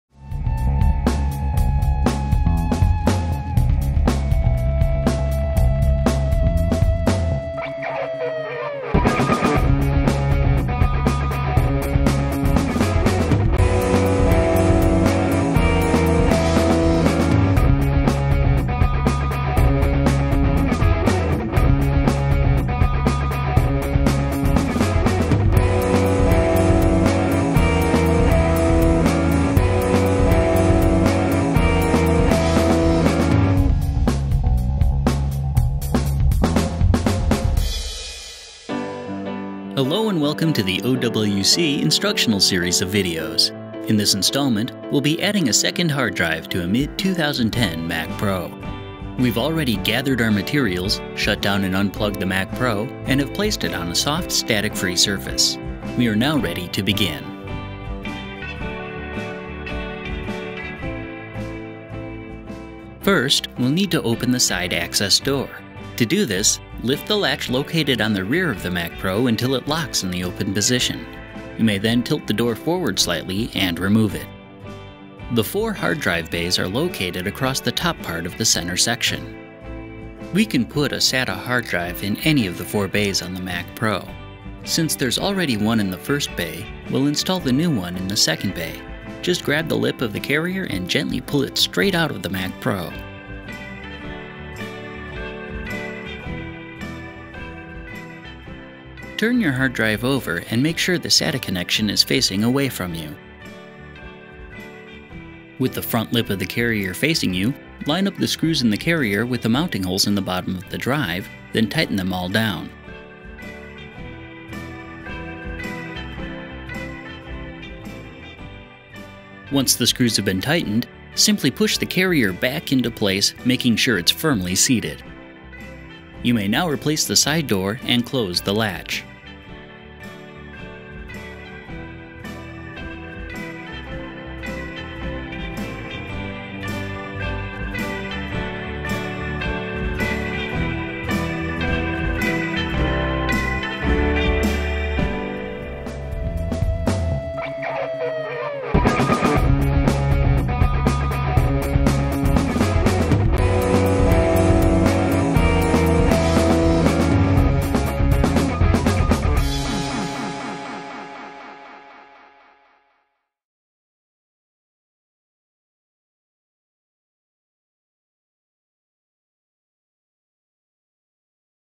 How to install a hard drive in a Mac Pro (Mid 2010) MacPro5,1
OWC's step-by-step video of how to install a hard drive in the Apple Mac Pro (Mid 2010) with Model ID: MacPro5,1.